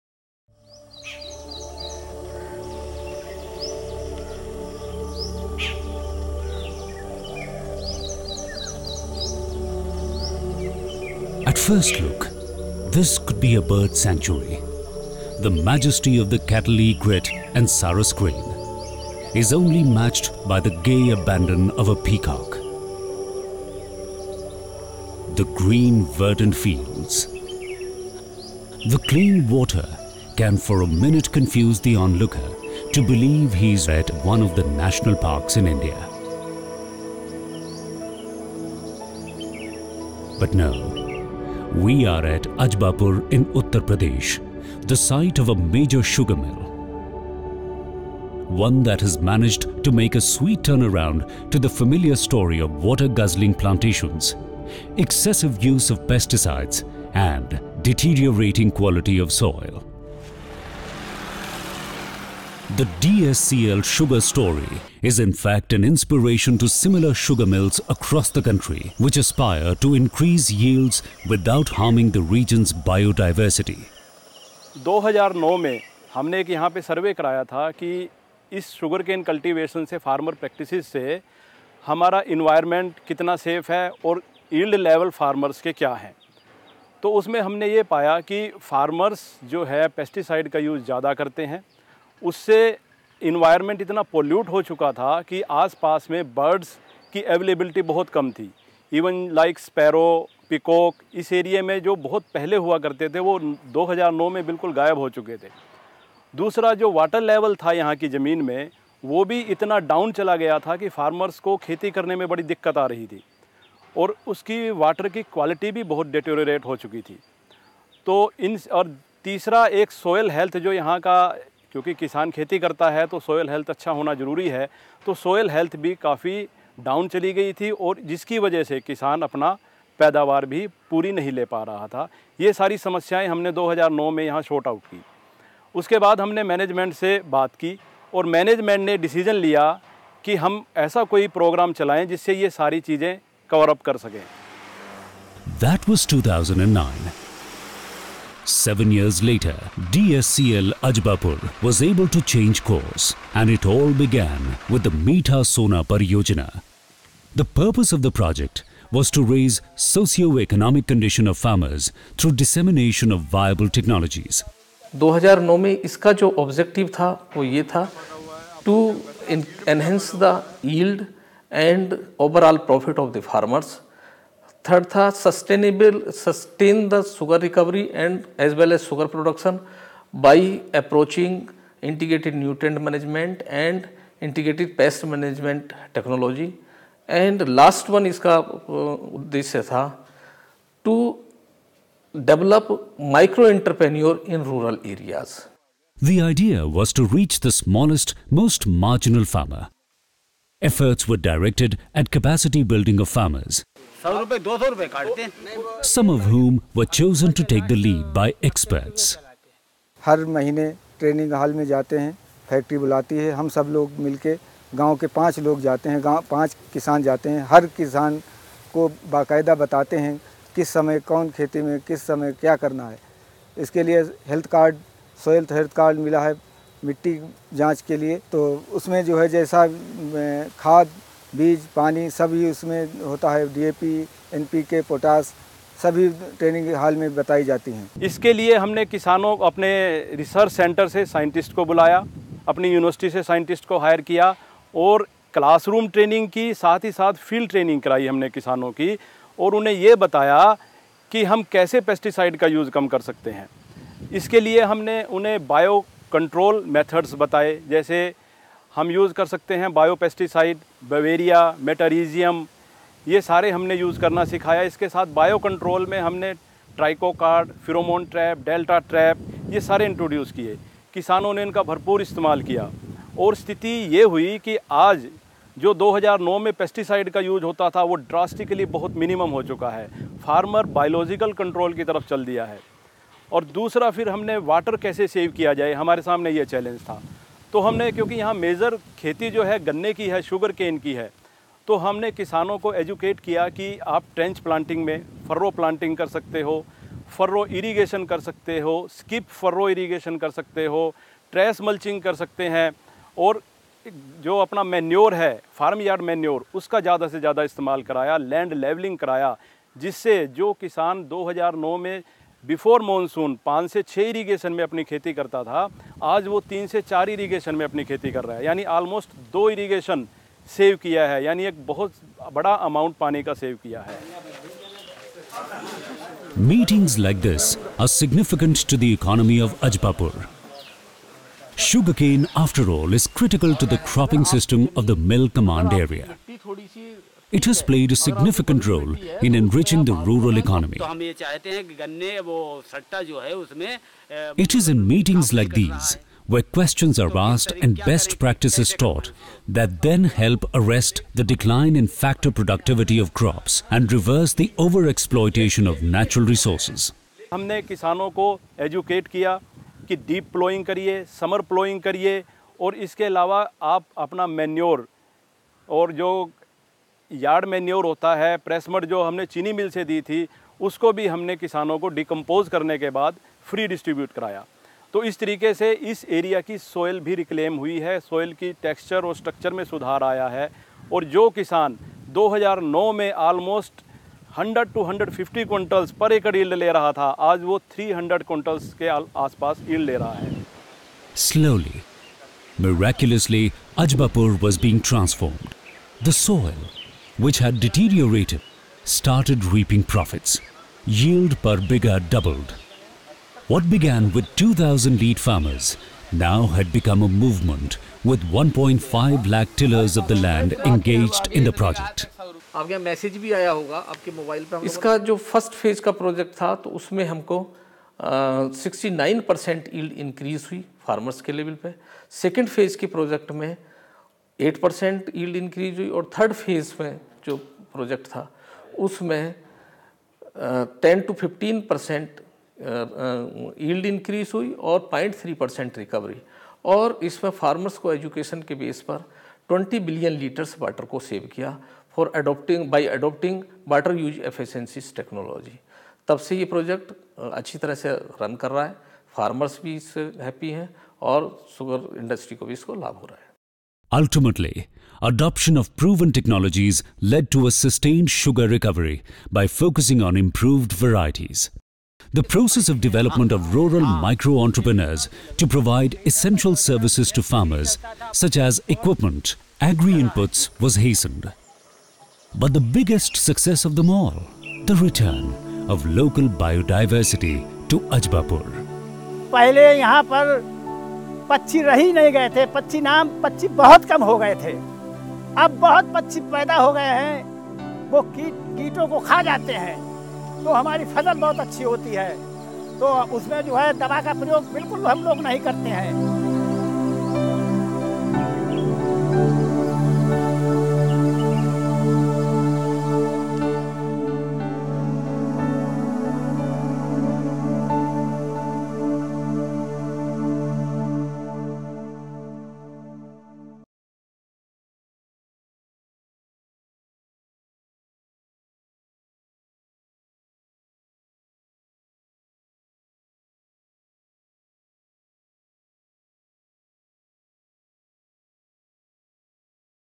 Biodiversity Initiatives. DSCL Sugar - Ajbapur
Biodiversity Initiatives - by DSCL Sugar, Ajbapur
Adoption of proven technologies led to a sustained sugar recovery by focusing on improved varieties.

The process of development of rural micro entrepreneurs to provide essential services to farmers such as equipments, agri-inputs was hastened, resulting in the biggest success.
The return of local biodiversity to Ajbapur.